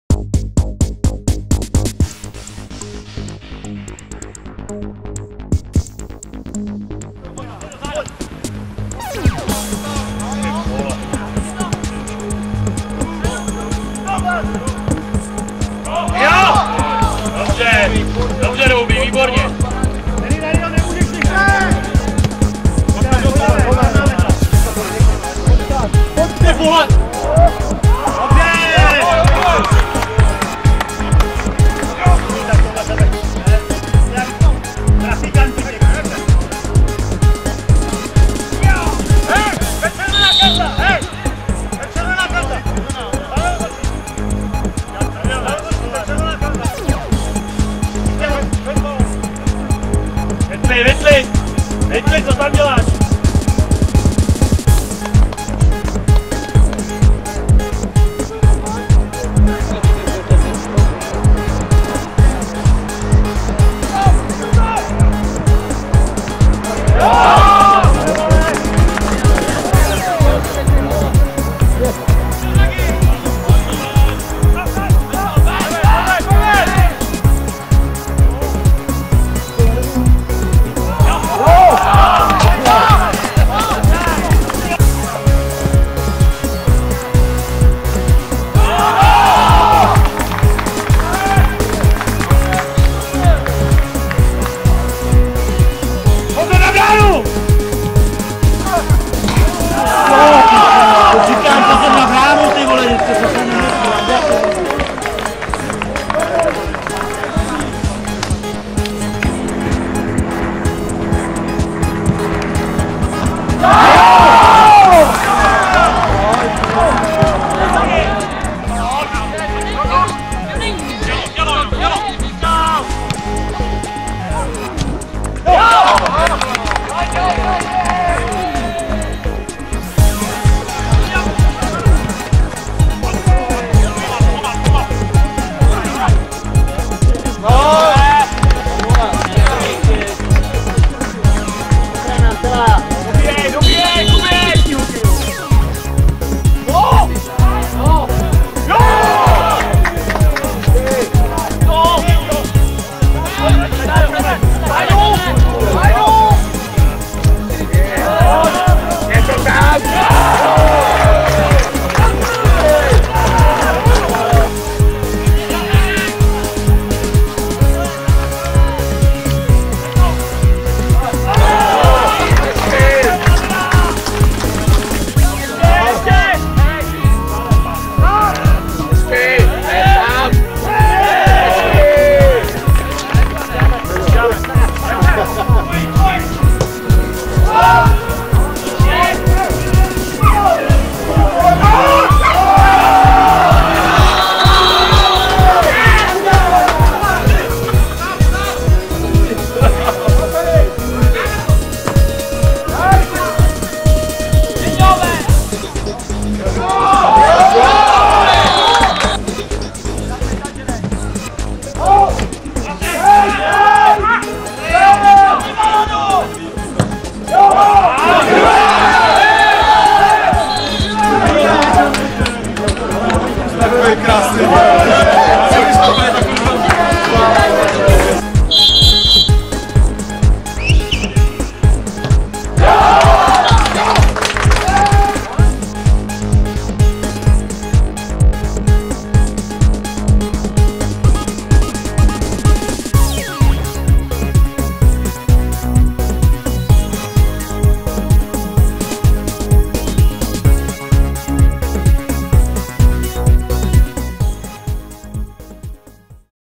Pražská teplárenská přebor mužů 2012/2013 14.kolo
Sestřih gólů 14.kola z klubových kamer 2